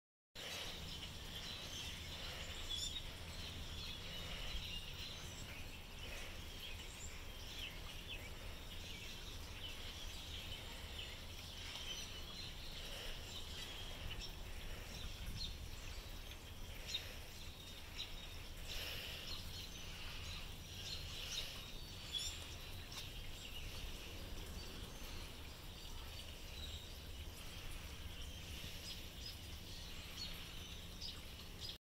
Birds are all over my lawn
There are so many birds over my lawn.